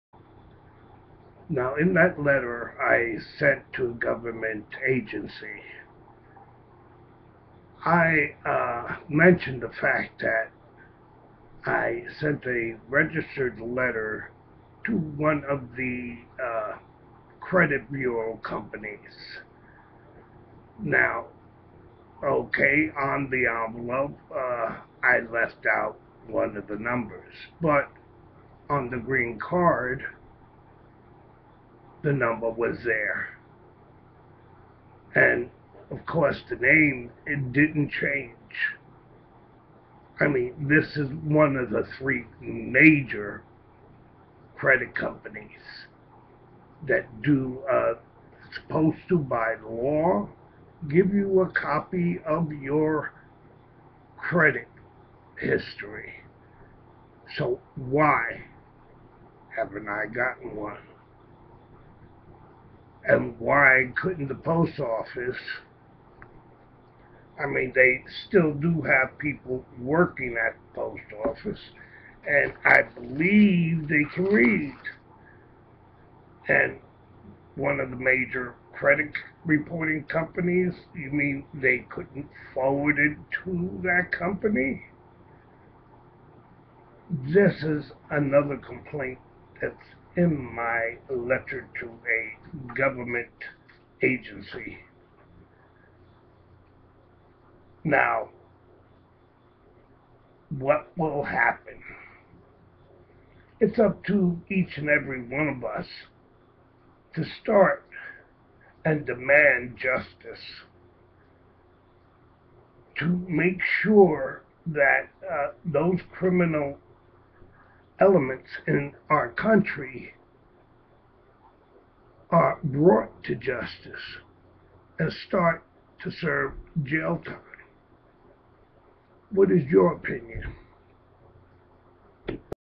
The United States post office can't deliver mail!
The United States post office can't deliver a certified registered return receipt requested piece of mail to one of the major credit reporting companies, why? And will the government agency that I have reported this too take action?